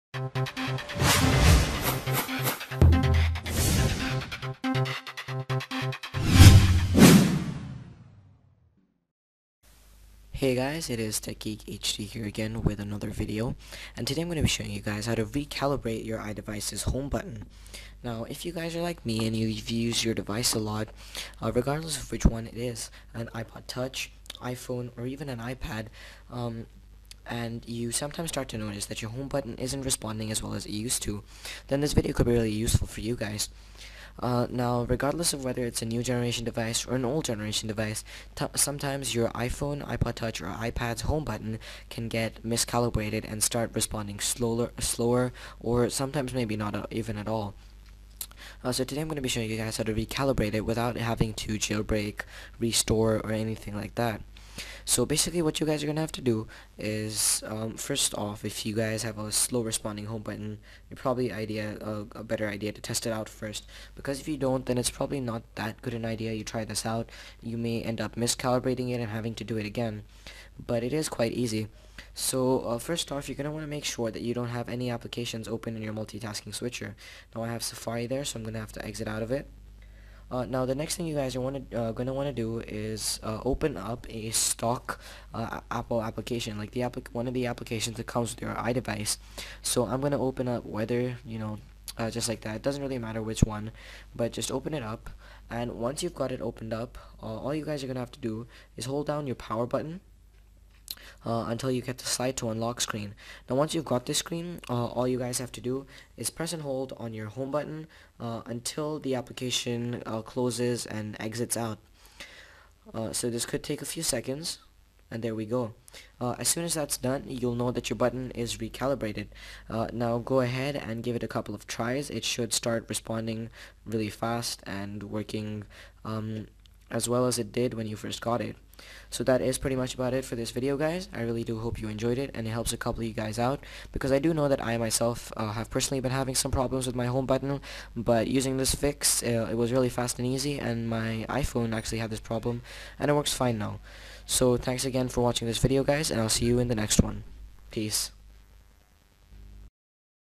Tutorial: How to Re-Calibrate your iDevices' Home Button.
It is a very simple and easy fix for a problem that occurs in may iDevices, i hope this video helps some of you out, if it does, i would really appreciate your subscription, and as always, have a great day.